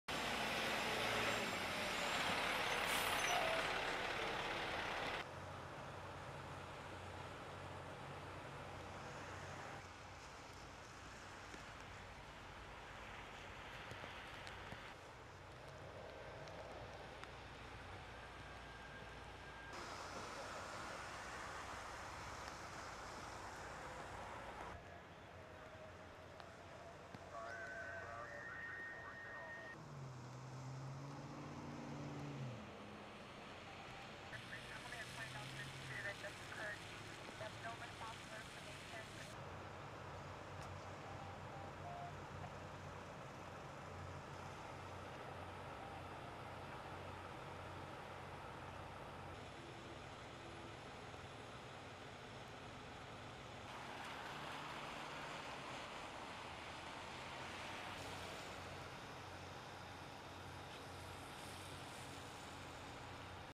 One Killed In Modesto Roll Over Traffic Accident
A vehicle rolls off of the Briggsmore/99 overcrossing, ejecting the occupant and killing them on impact.